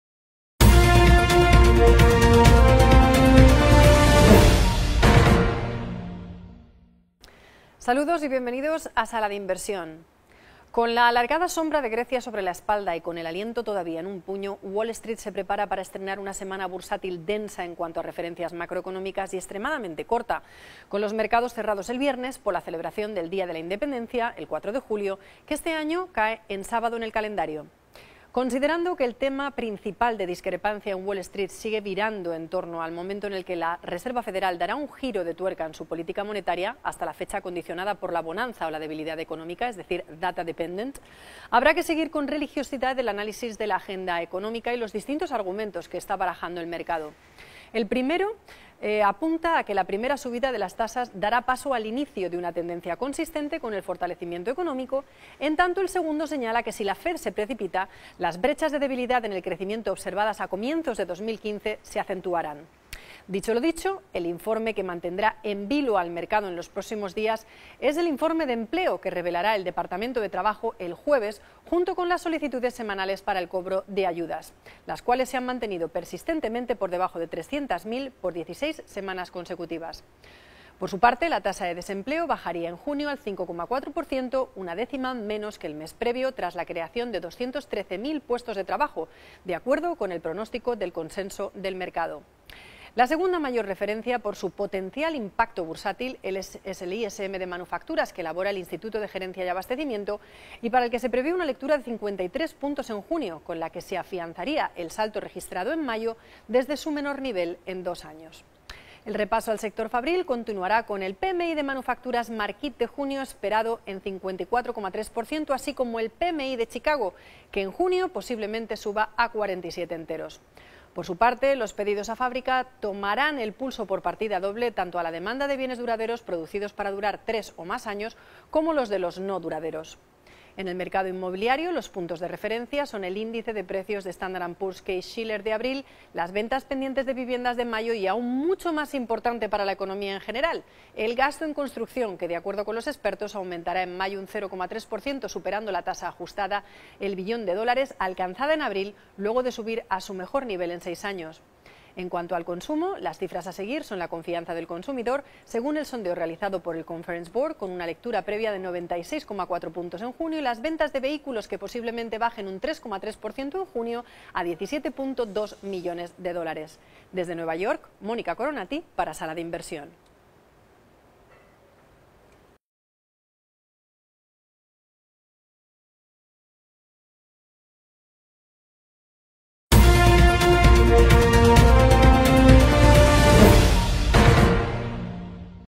Agenda semanal: sin fuegos artificiales en Wall Street
Con la alargada sombra de Grecia sobre la espalda, y con el aliento todavía en un puño, Wall Street se prepara para estrenar una semana bursátil densa en cuanto a referencias macroeconómicas, y extremadamente corta, con los mercados cerrados el viernes por la celebración del día de la Independencia, el 4 de Julio, que este año cae en sábado en el calendario.